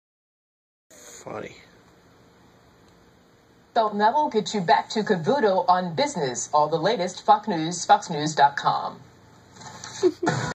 Fox News Anchor DROPS the F-word on live TV
Fox News Anchor uses the F-word on live TV, drops the F bomb on live tv, saying F--- NEWS,